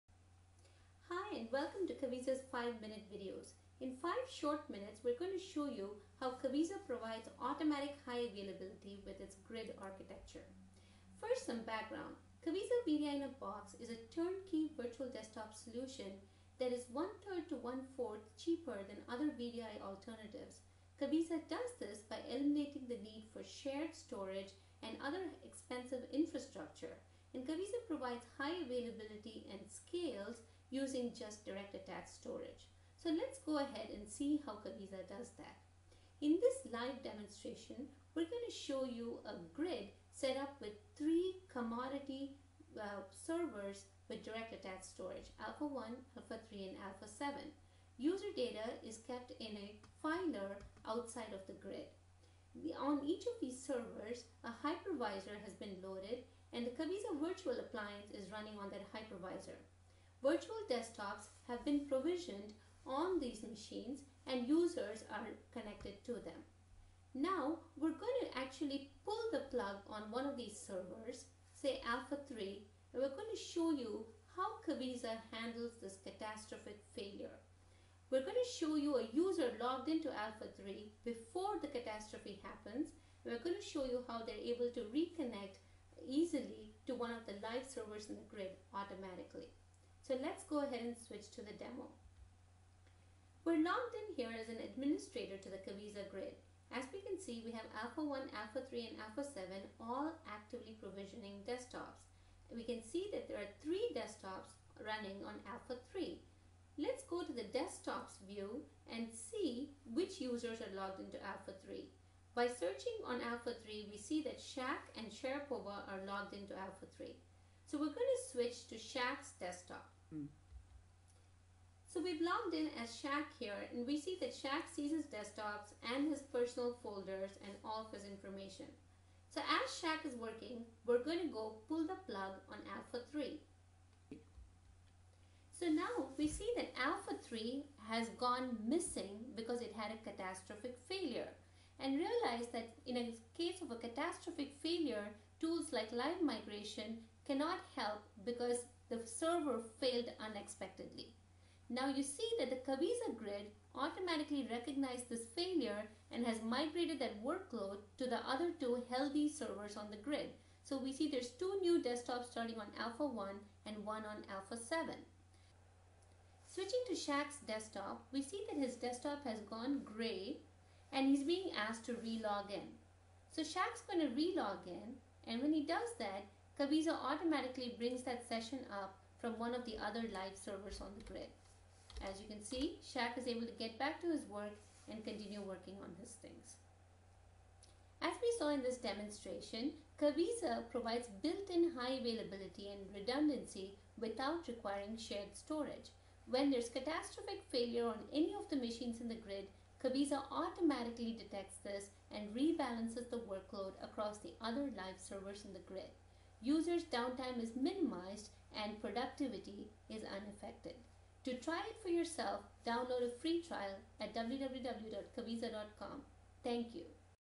Kaviza's built-in high-availability in action
See how Kaviza's built-in high-availability works -we pull the plug on one of the virtual desktop servers in a Kaviza grid and see how Kaviza automatically recovers from this catastrophic failure.